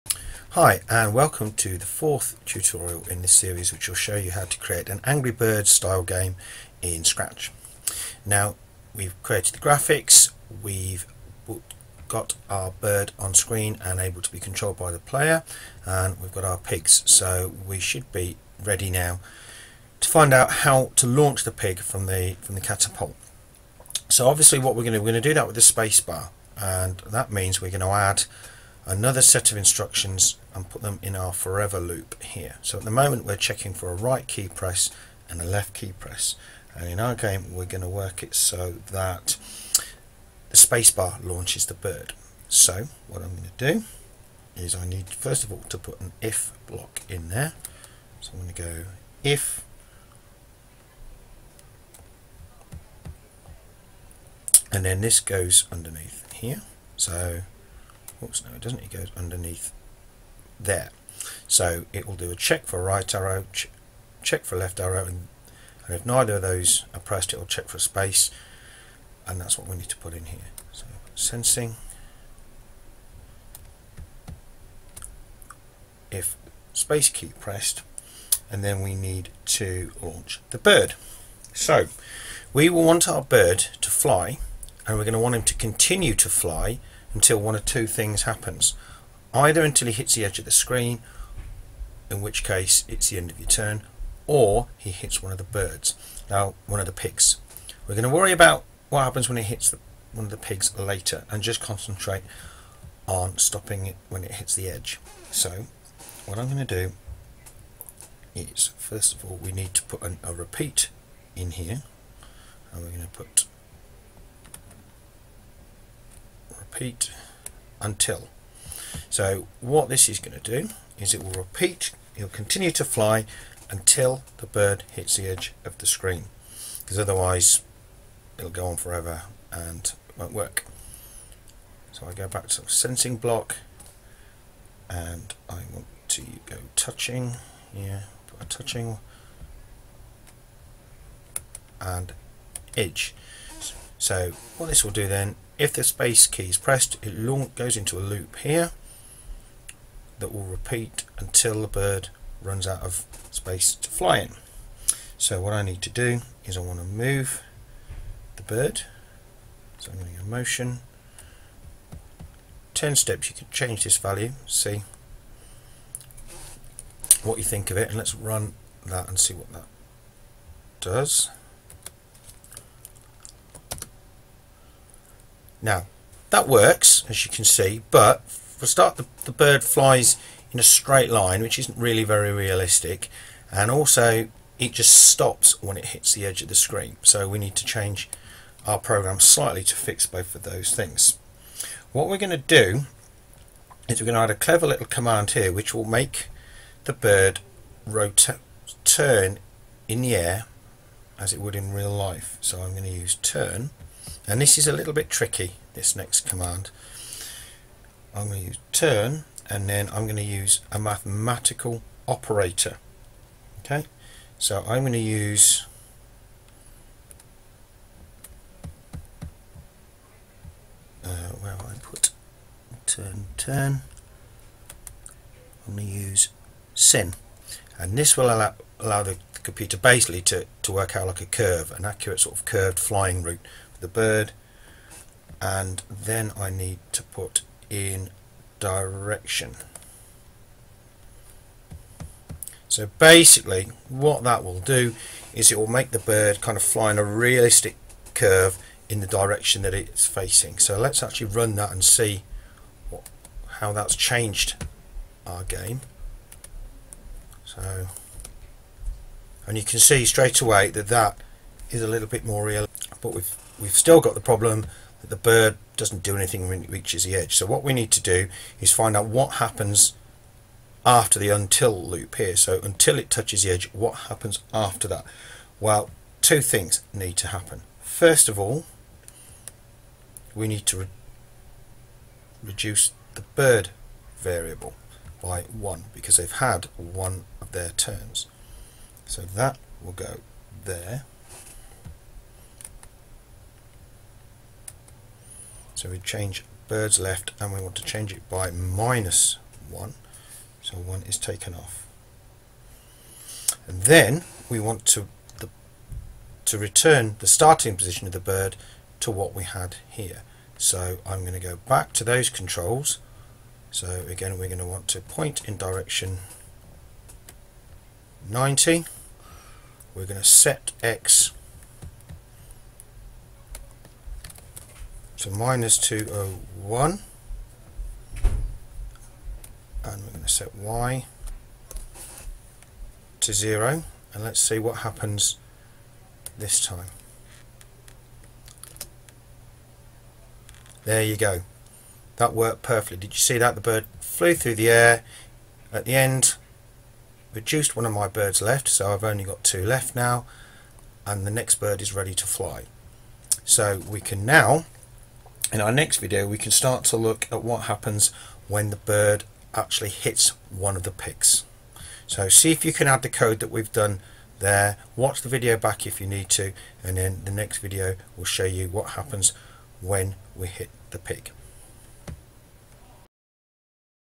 Angry Birds Scratch 5: Launching & moving the bird
This is the fifth video in a series, showing how to create an Angry Birds style game in Scratch.

In this video you will learn how to program the game so that the player can launch the bird, make it fly in a realistic way, and stop when it hits the edge of the screen